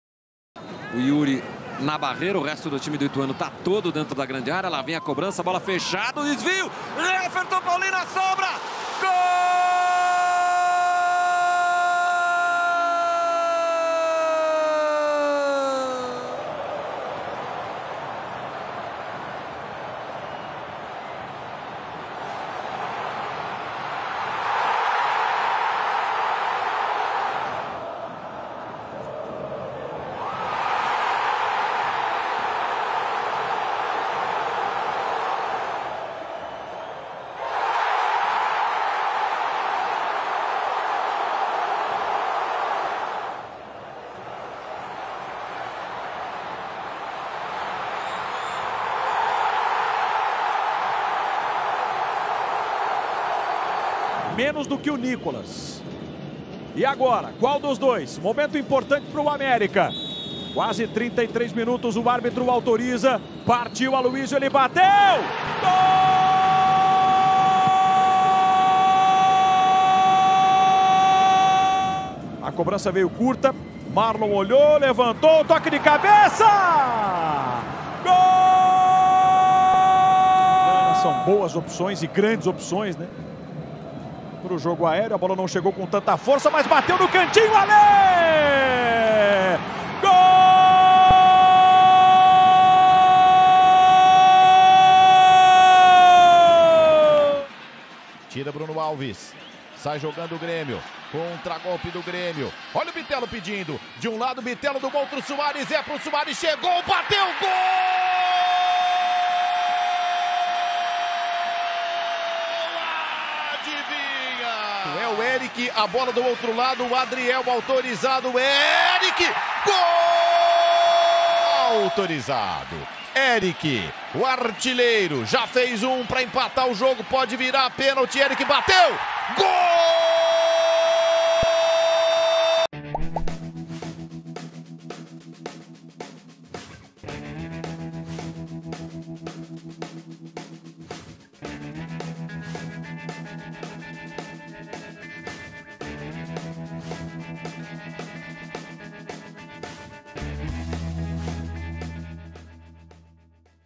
GOLS DO DIA 19/03/2023 | LISTAS | ge.globo
Veja os golaços dos Estaduais 2023:

⚽PALMEIRAS 1 X 0 ITUANO
⚽VASCO 1 X 3 FLAMENGO
⚽AMÉRICA-MG 2 X 1 CRUZEIRO
⚽YPIRANGA 2 X 1 GRÊMIO